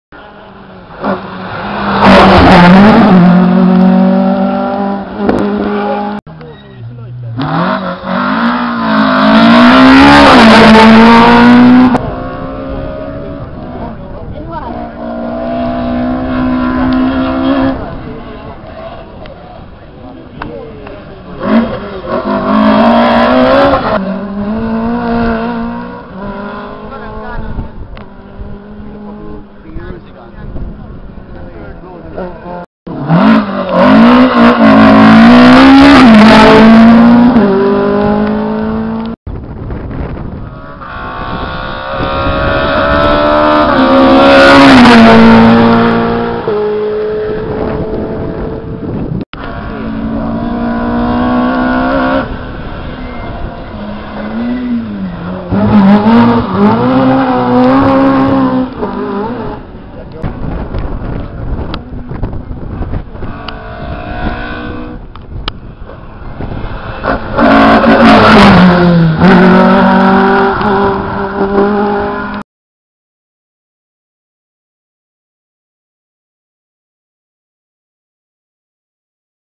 Charlie and Johnny Hickey Rally Of The Lakes 2011
Geat rally by the Hickey crew ...Up to 3rd overall in International ahead of a WRC and all the GP N's Heading into final stage.